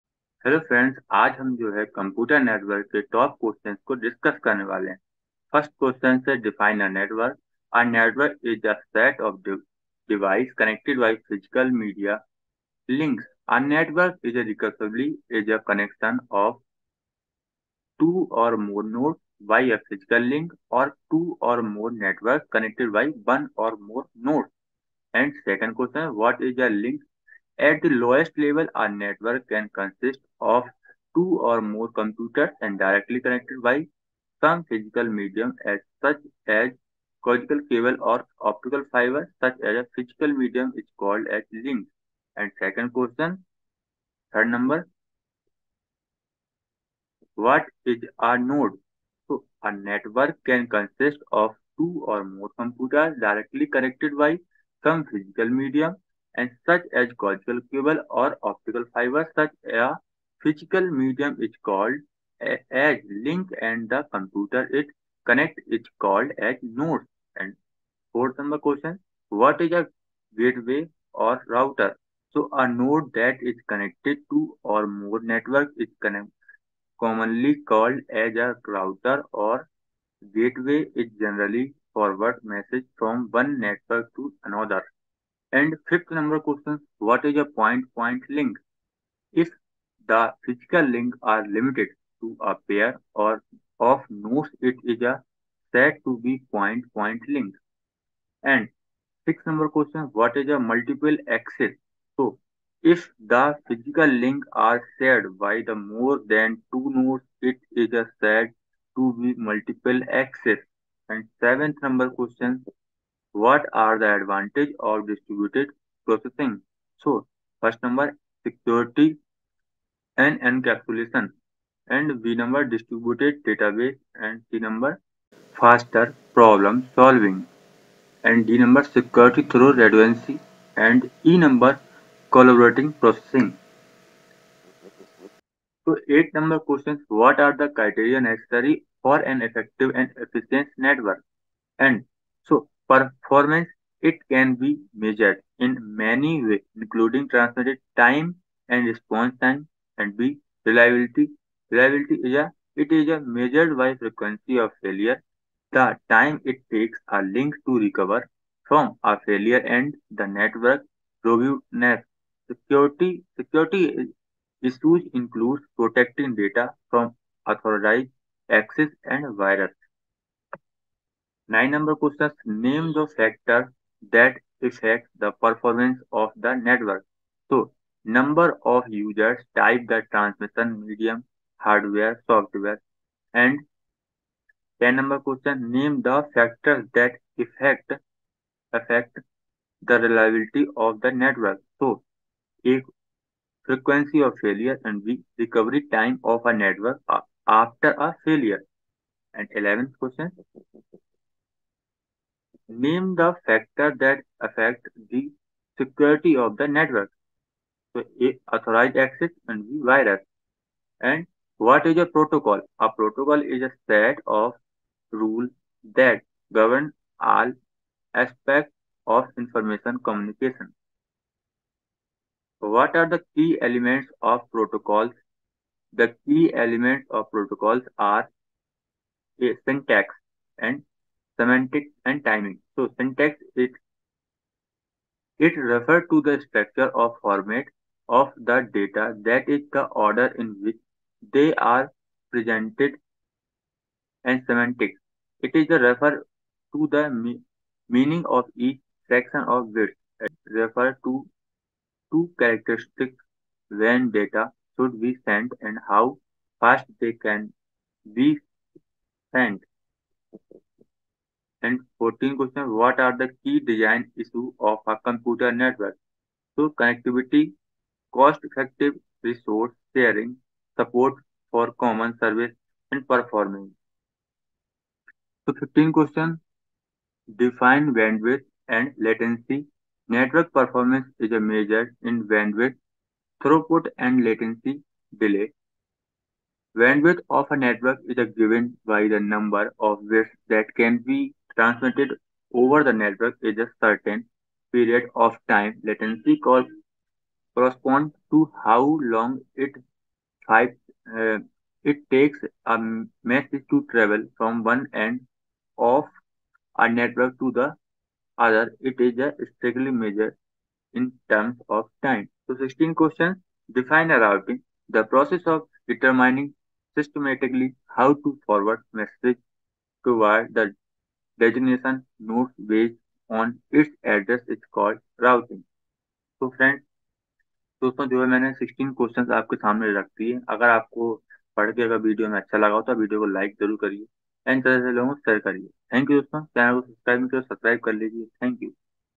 Top 16 Computer Network Viva Questions Last Moment Tuitions by kuldeep chaurasia
What is the network?
 What do you mean by network topology?
What are the advantages of Distributed Processing?
What is the criteria to check the network reliability?
Which are the different factors that affect the security of a network?